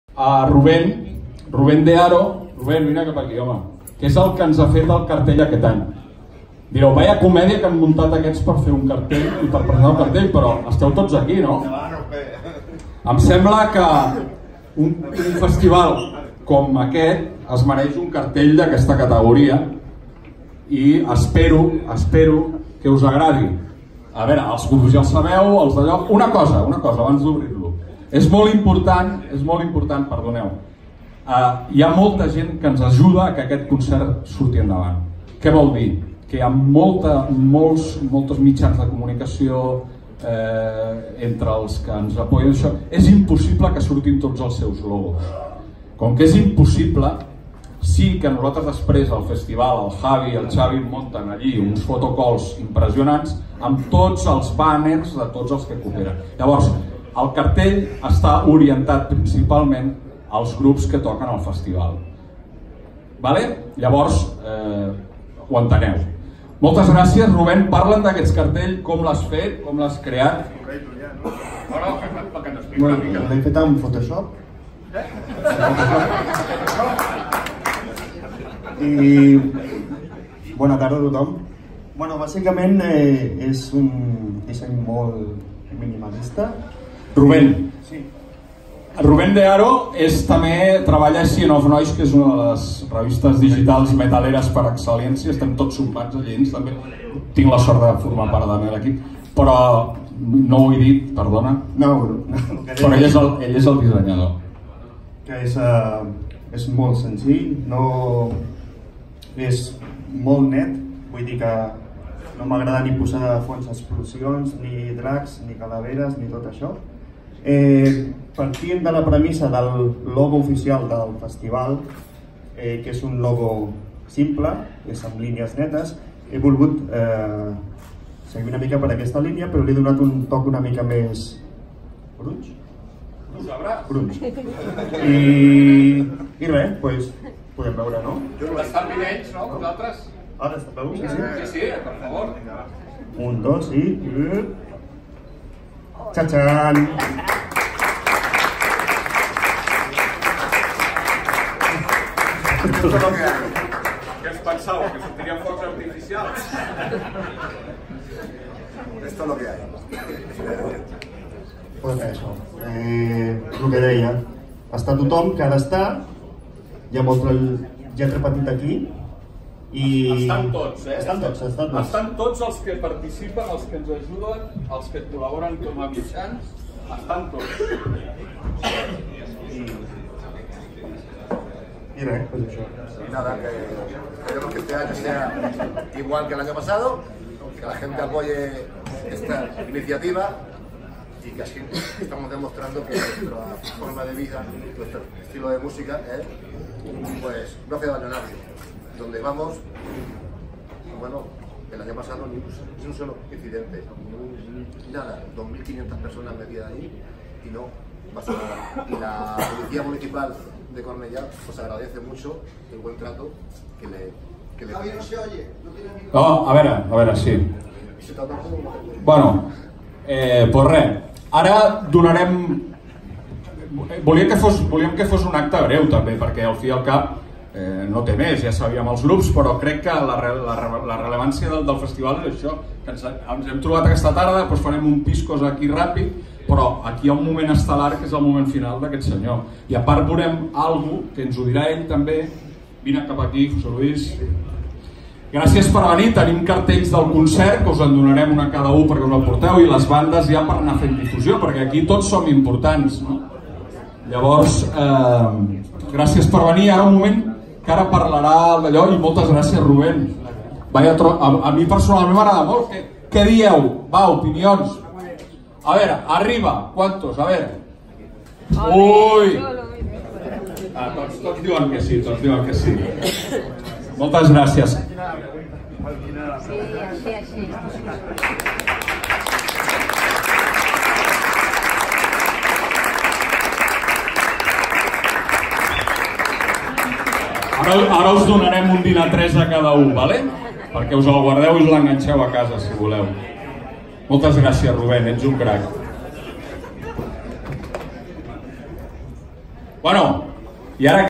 Presentación Can Mercader festival 2019 en el Fnac Arenas
MUESTRA DEL CARTEL OFICIAL

cämara Héctor guillermo Izuel Fernandez
FRIKITEAM